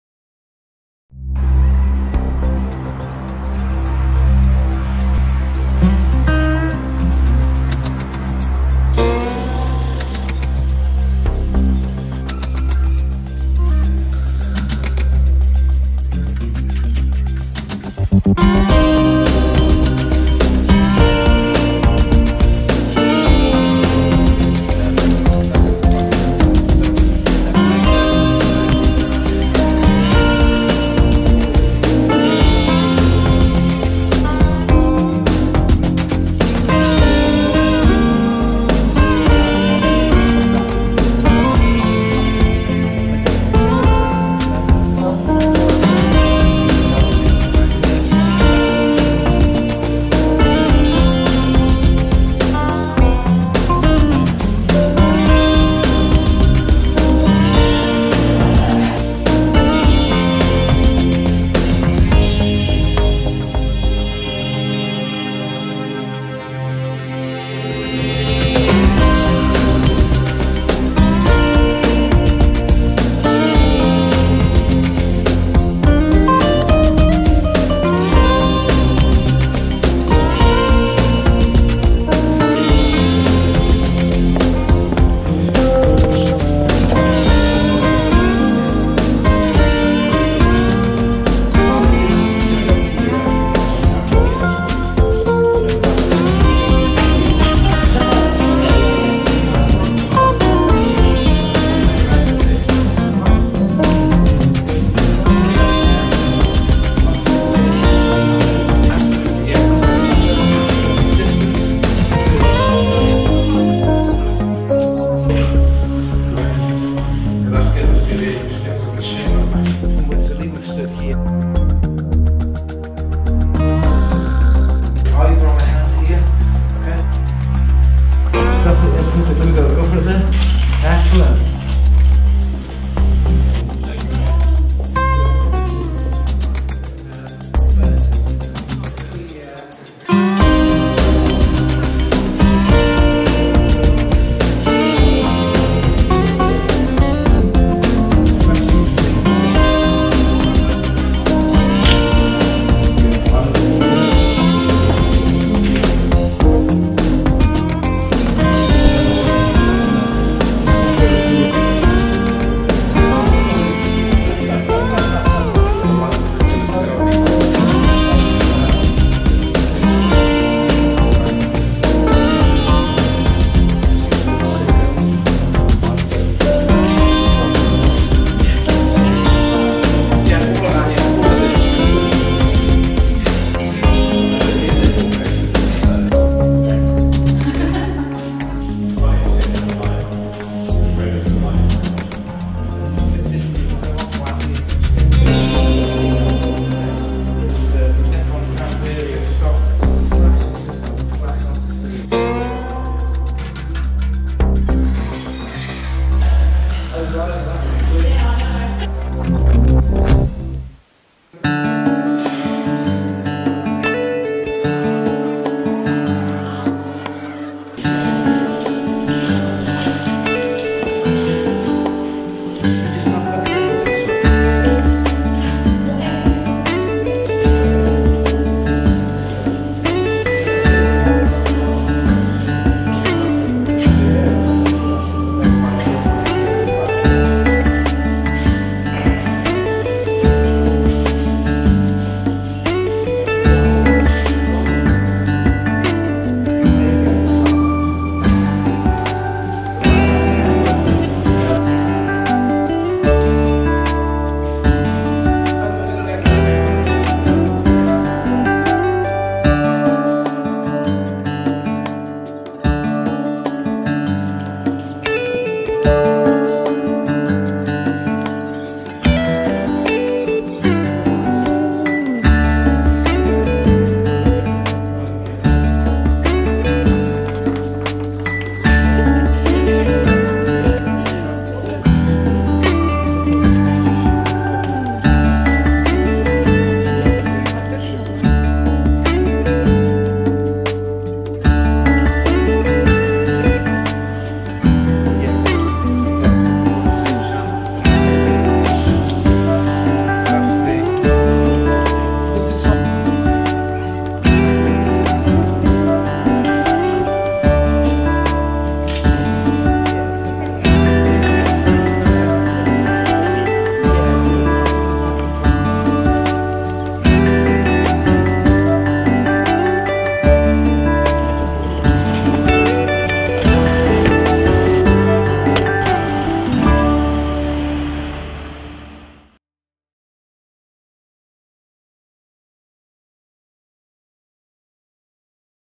The Societies 2013 Convention Videos - Richard Walton
Richard Walton London photographic Convention and Trade show location shoot - the event takes place each January and attracts thousands of photographers